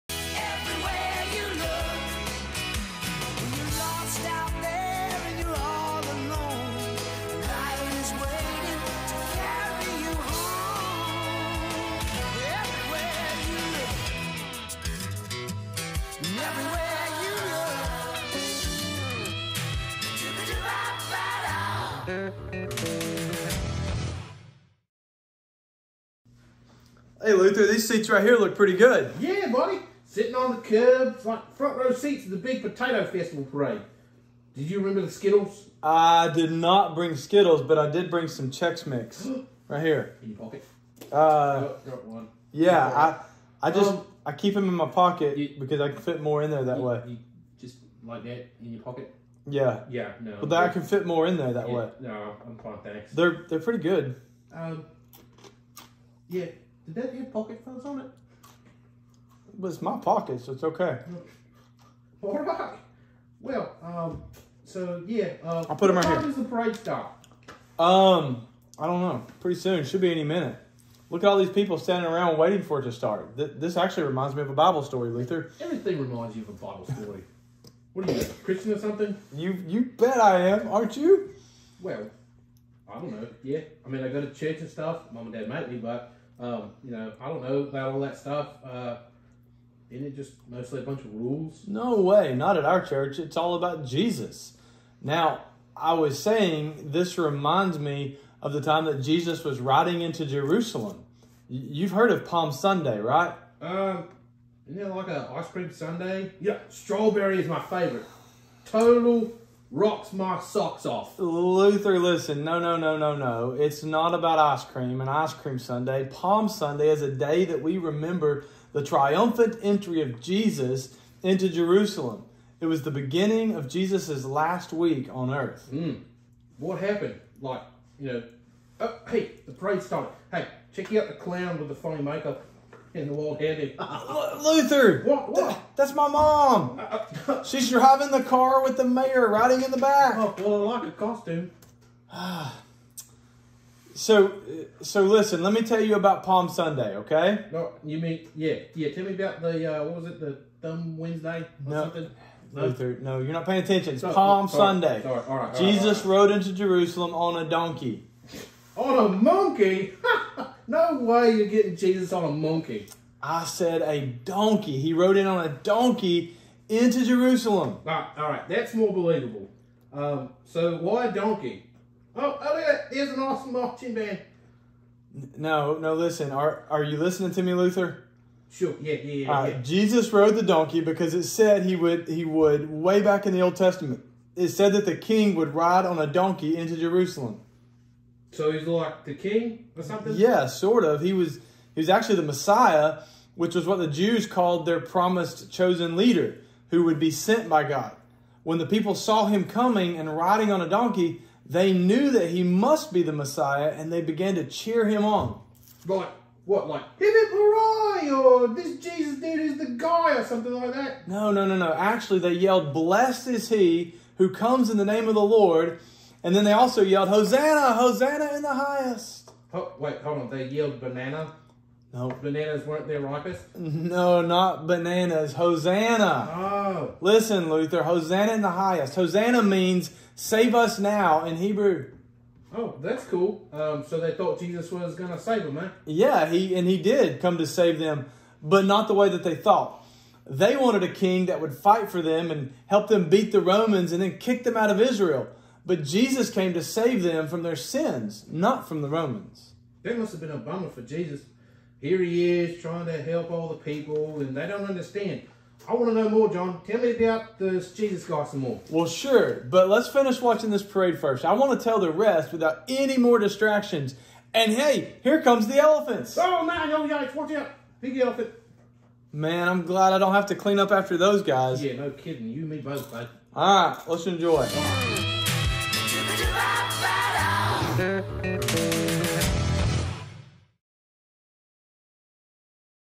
Table Talk With Luther Season 2- Episode 14: Palm Sunday
Yesterday was Palm Sunday, so today Luther & Pastor J are talking about what Palm Sunday means.